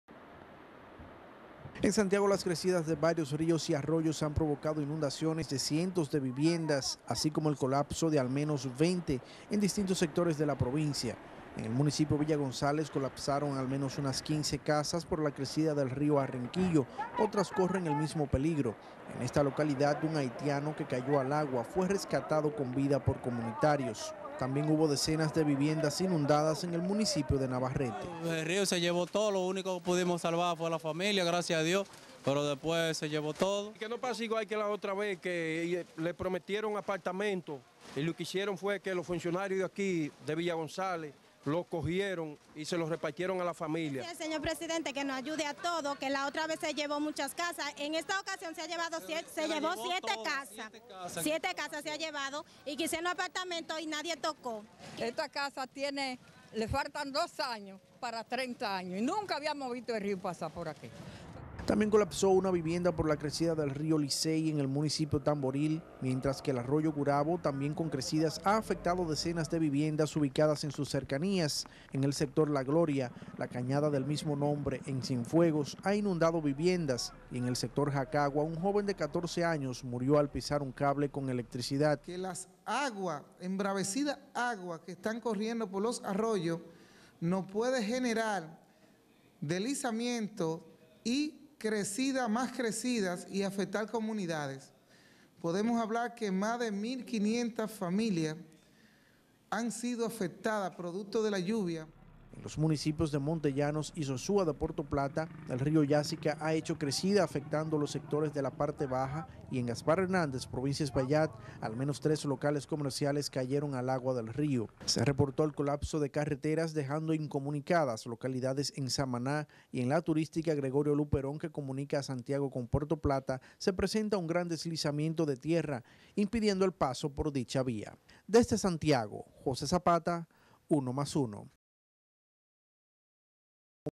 Las lluvias causan numerosos daños en el Cibao
Miles de viviendas inundadas decenas destruidas, así como miles de desplazados han sido parte de los daños causados por las crecidas de ríos en varias provincias del Cibao como consecuencia de una vaguada ubicada en la parte Norte, la cual ha causado lluvias continuas en las últimas 48 horas.